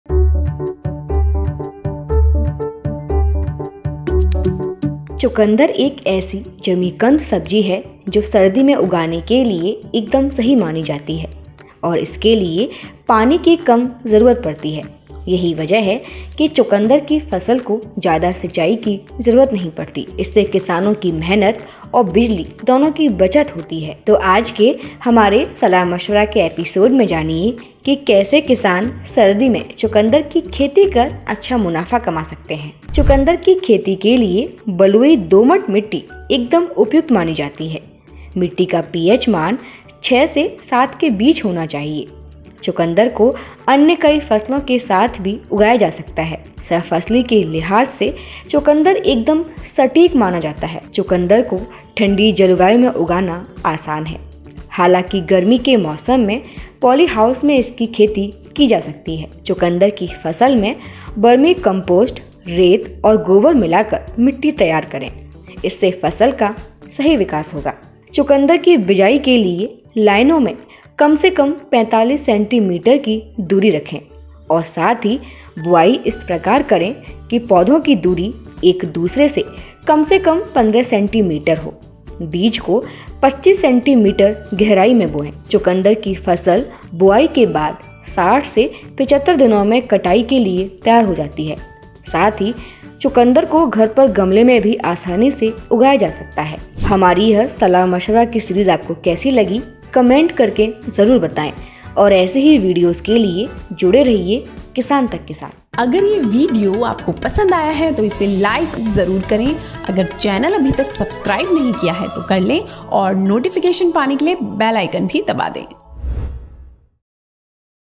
Farming Tips: Beetroot की खेती के लिए Farmers अपनाएं ये आसान Tips | Kisan Tak
चुकंदर एक ऐसी जमीकंद सब्‍जी है, जो सर्दी में उगाने के लिए एकदम सही मानी जाती है और इसके पानी की कम जरूरत पड़ती है. यही वजह है कि चुकंदर की फसल को ज्‍यादा सिंचाई की जरूरत नहीं पड़ती. इससे किसानों की मेहनत और बिजली दोनों की बचत होती है.तो आज के हमारे सलाह मशवरा के इस एपिसोड में जानिए कि कैसे किसान सर्दी में चुकंदर की खेती कर अच्छा मुनाफा कमा सकते है.